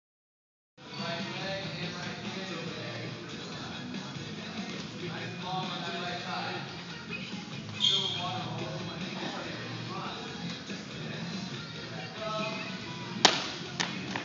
Medball clean demo
Not a fan of this movement as a tool to teach clean, but great for conditioning workouts